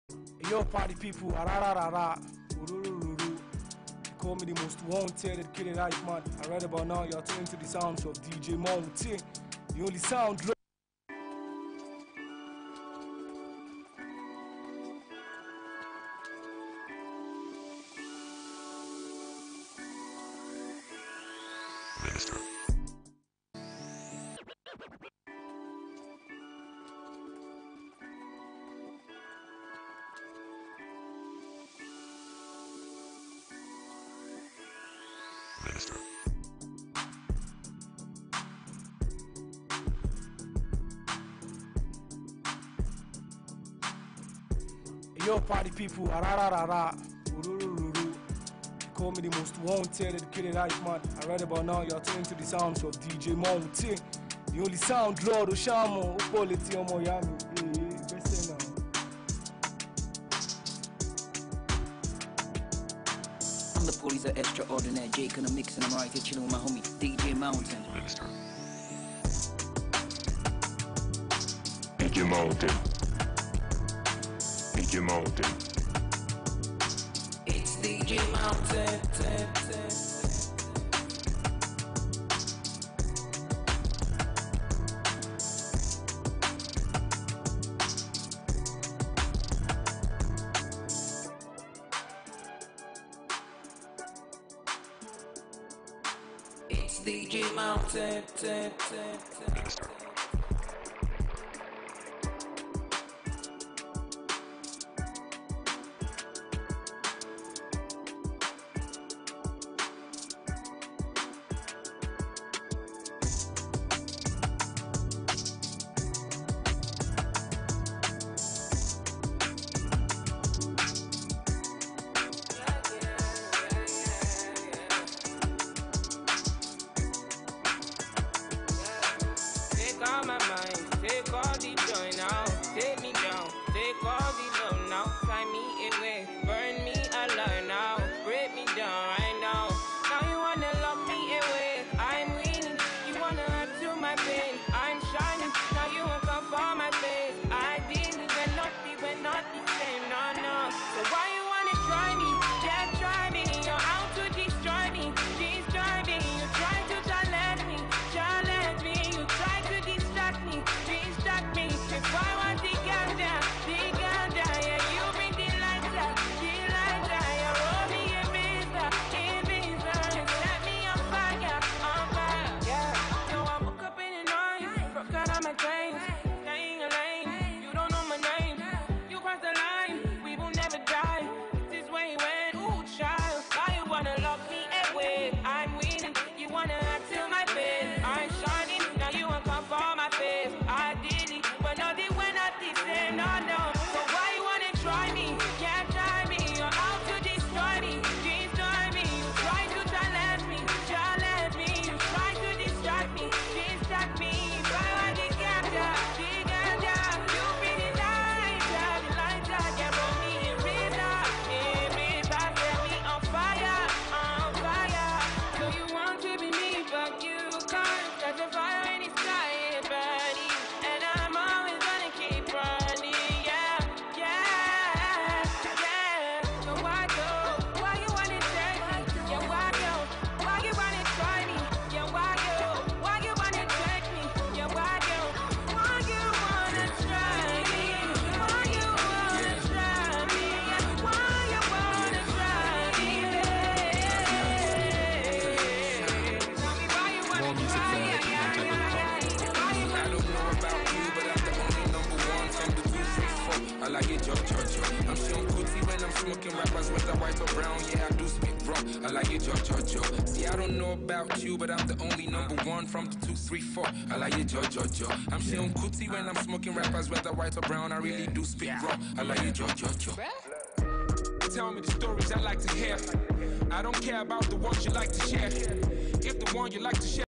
OneEkiti Twitter Party
2019 ONE EKITI TWITTER  PARTY LIVE IN ADO-EKITI AT ADUNNI OLAYINKA CENTER FAJUYI ADO-EKITI STATE, NG.